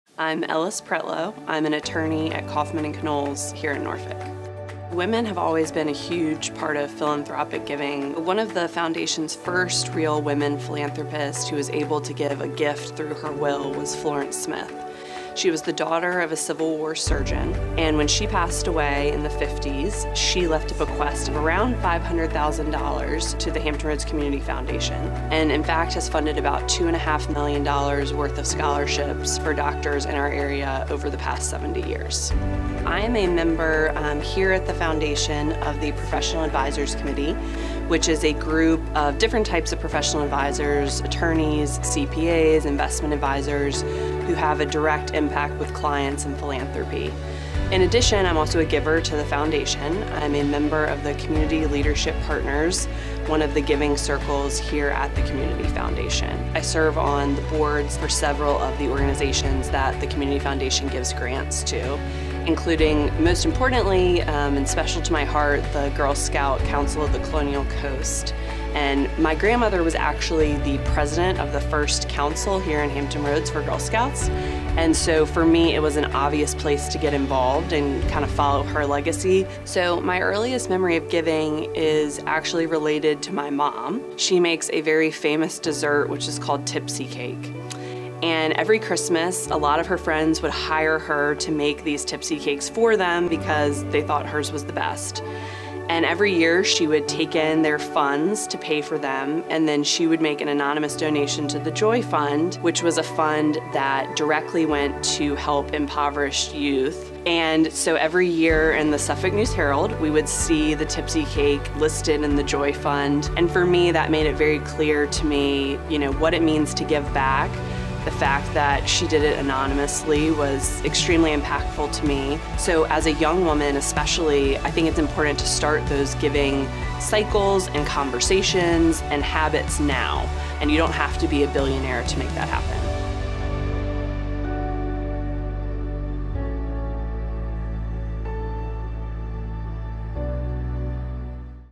Attorney Ellis Pretlow Celebrates Women Philanthropists
Women are philanthropic powerhouses. Their efforts support communities in need and an array of charitable causes. Attorney Ellis Pretlow discusses some amazing women who have made a lasting impact in Hampton Roads, including causes closest to her heart. She offers advice about how girls and young women can get involved in giving back even at an early age.